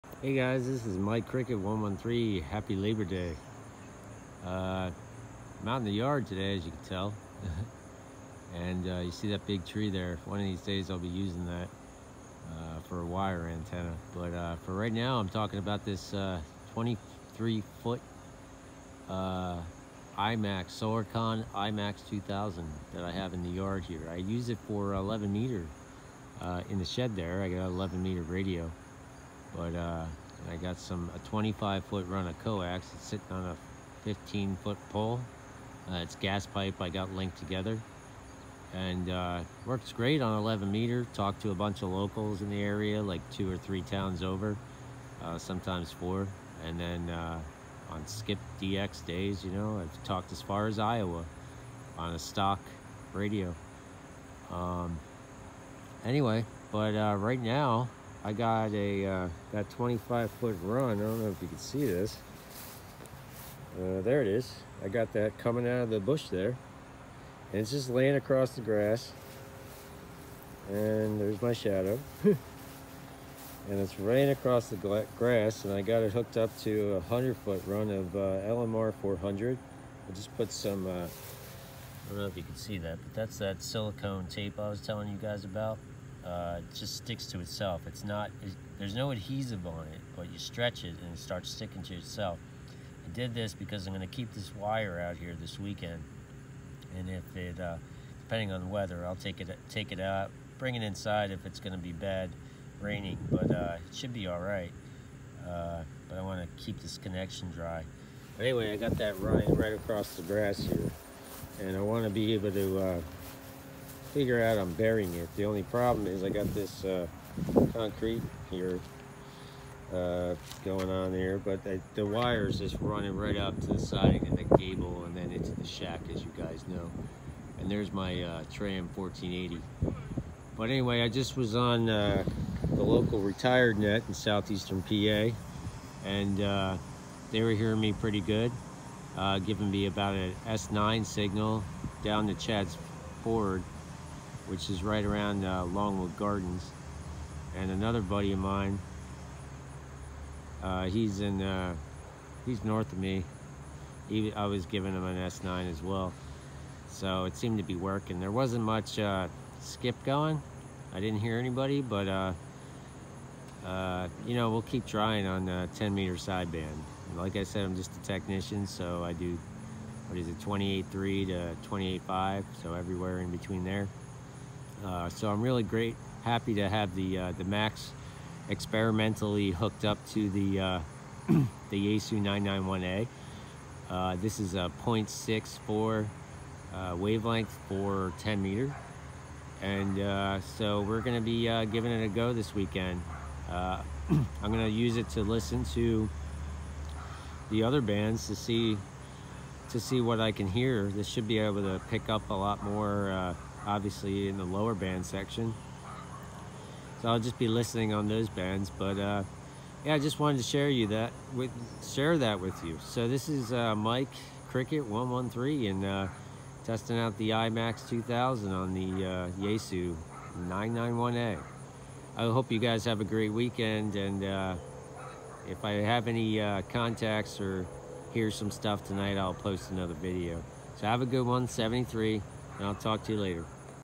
Imax 2000 on 10 meters
I hooked my Imax 2000 up to my Yaesu FT-991a.   Swr was near perfect.  Really happy with the results. Checked into local net on 28.435 USB. Every day at 1 pm.  Signal reports were S9-10 from 15 or so miles away.  Received is great too.